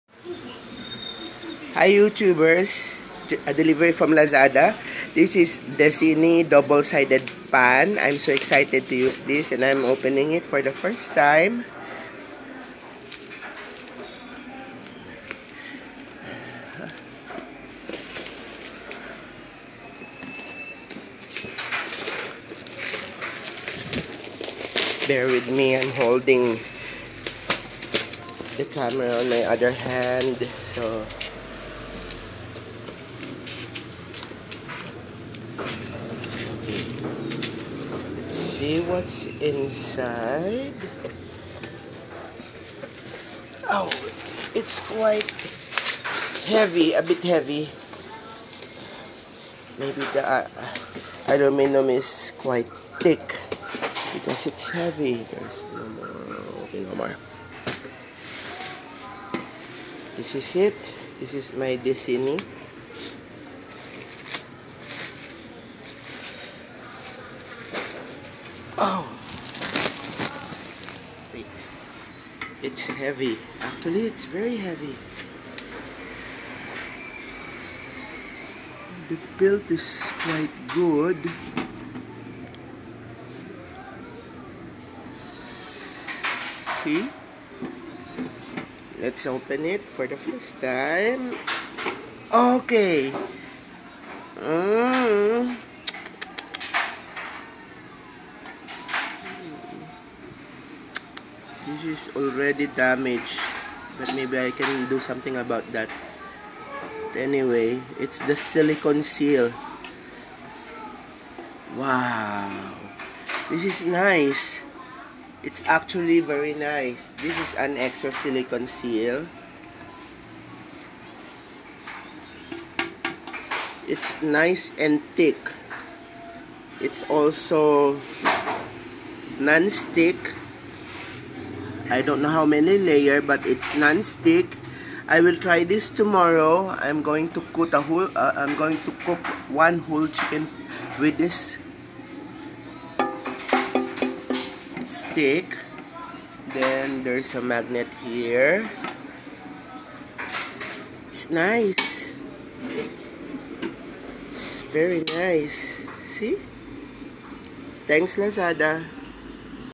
Dessini Italy Double Sided Grill Pan 36cm, from Laada.com
the extra silicon was badly placed, its totally damaged.